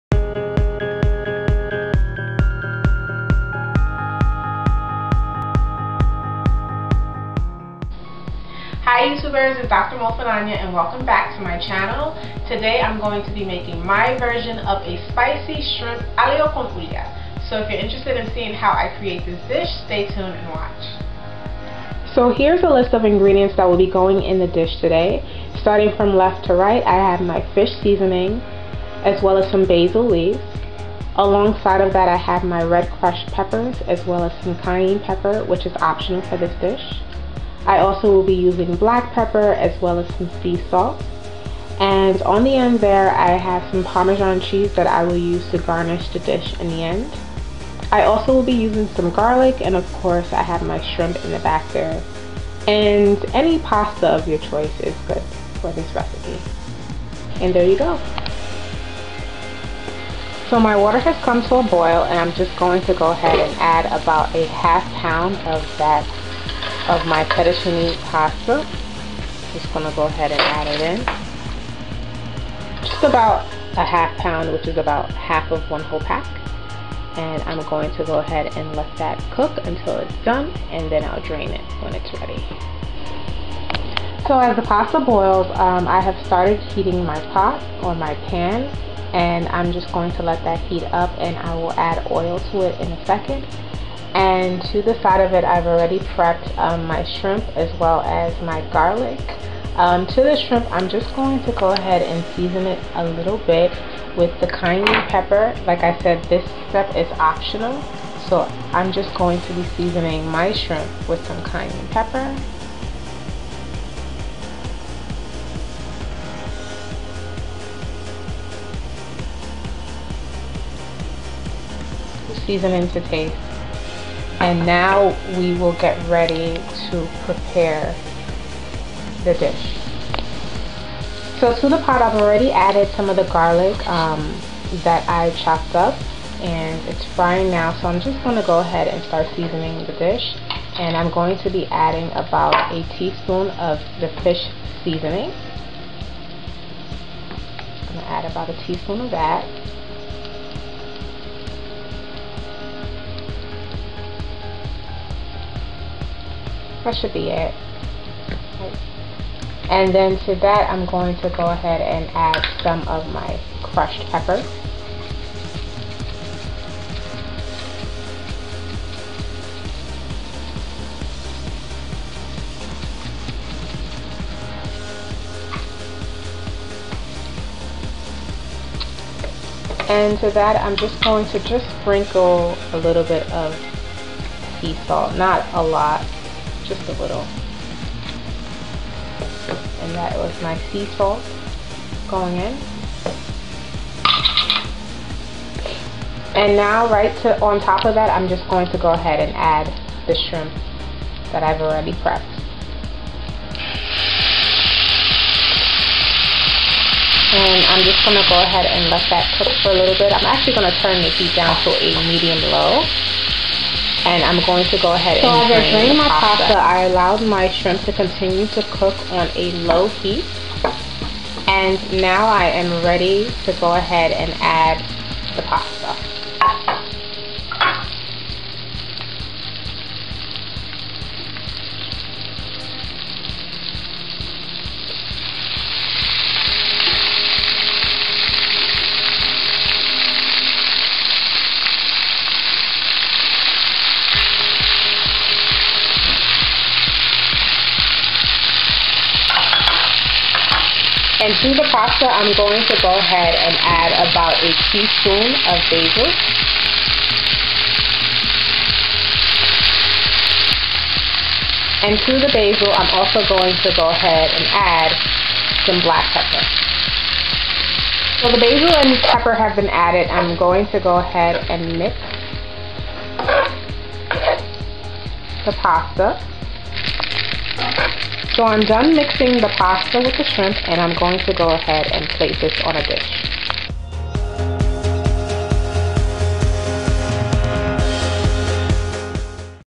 Spicy Shrimp Alio Olio
If your are looking for a really simple and delicious dinner recipe, you will enjoy this spicy shrimp with garlic and oil. It takes about 5 minutes to prepare, and dinner will be served in no time! Enjoy!!!

Ingredients:
shrimp
1 whole garlic bulb
1-2 tsp Fish seasoning
1-2 tsp Basil
cayenne pepper to taste, optional
salt & pepper to taste
crushed red peppers, sparingly
oil, to cover surface of the skillet
pasta of your choice
parmesan cheese to garnish